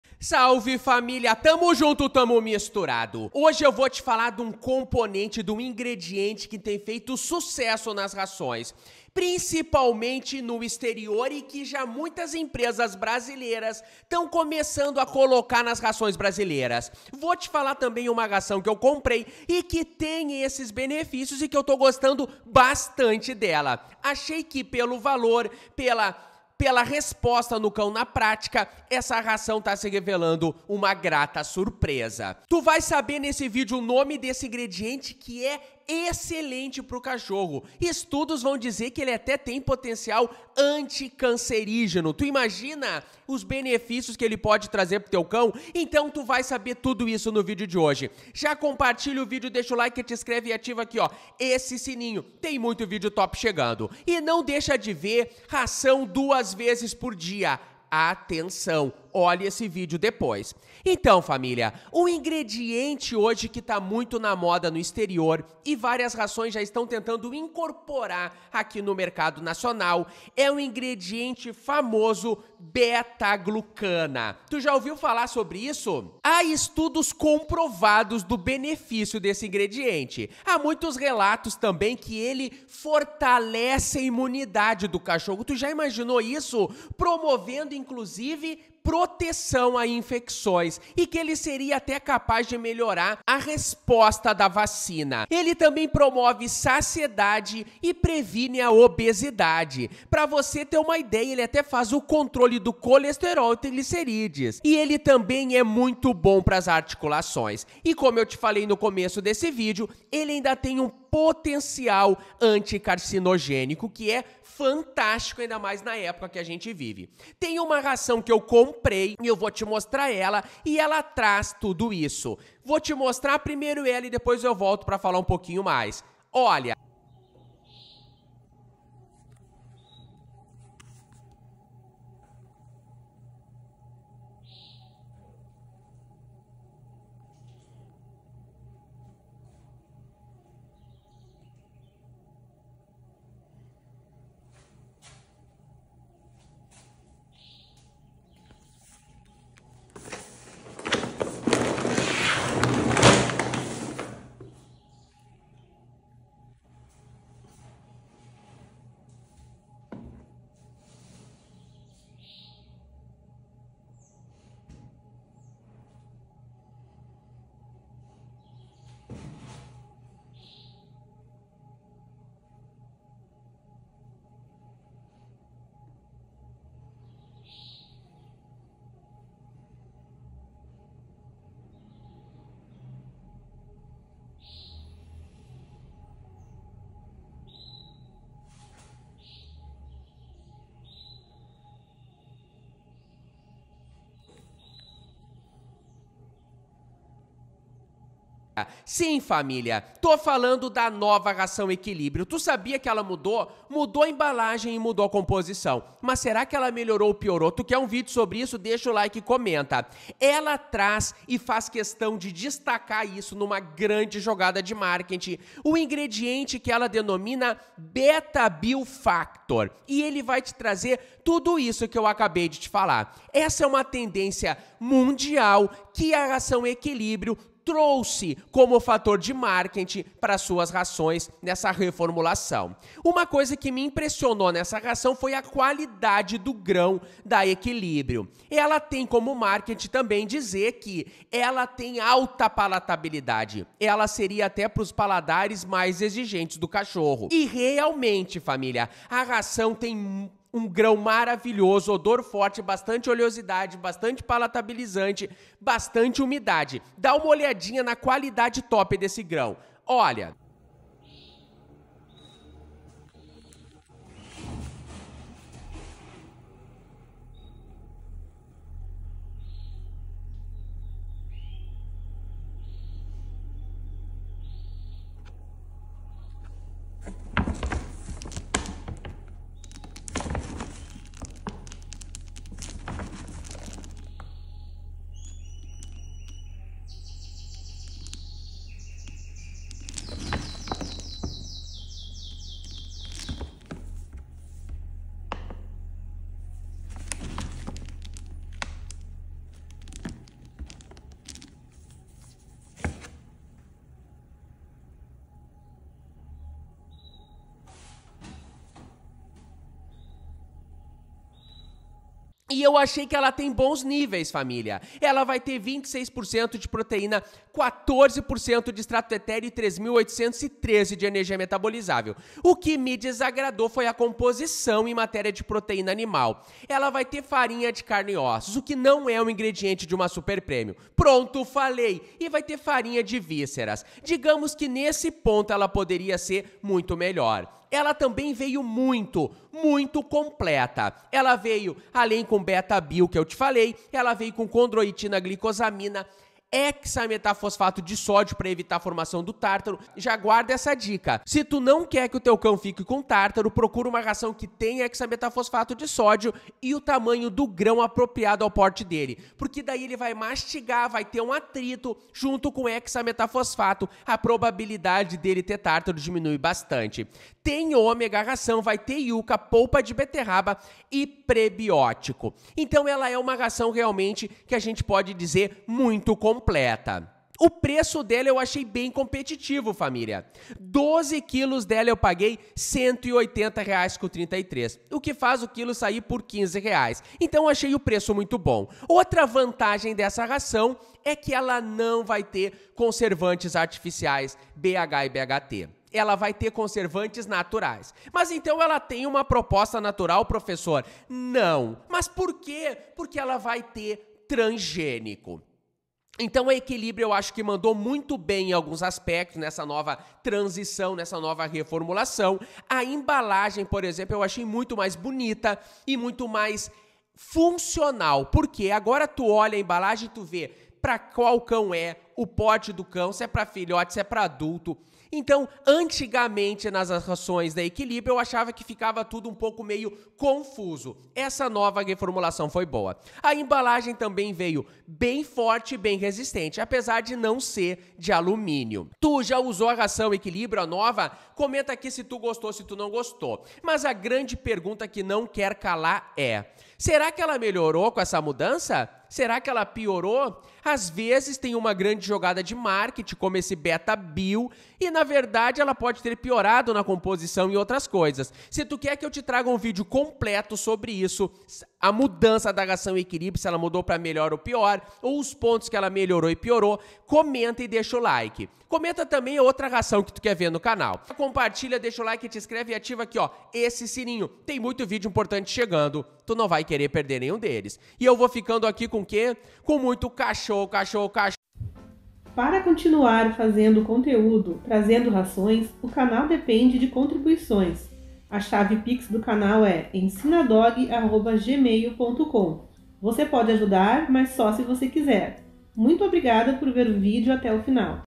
Ração SUPER PREMIUM ✅SENSACIONAL essa novidade!! | Cães e gatos
Ração SUPER PREMIUM ✅SENSACIONAL essa novidade!! | Cães e gatos Nesse video eu falo sobre uma ração super premium para cachorro e comento uma novidade sobre rações para cães e gatos.

👉 COMPRAR CURSOS